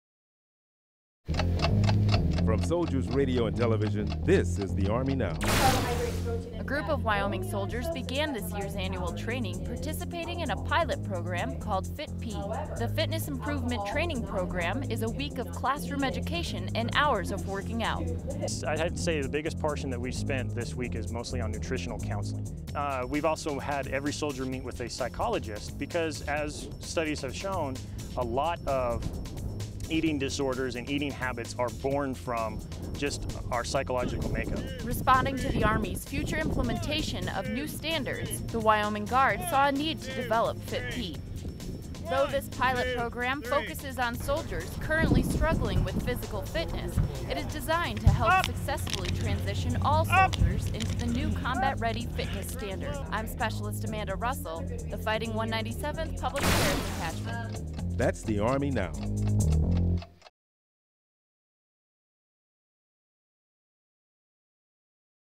Wyoming National Guard Soldiers Take Part In Summer Fitness
Wyoming National Guard Soldiers take part in summer fitness program.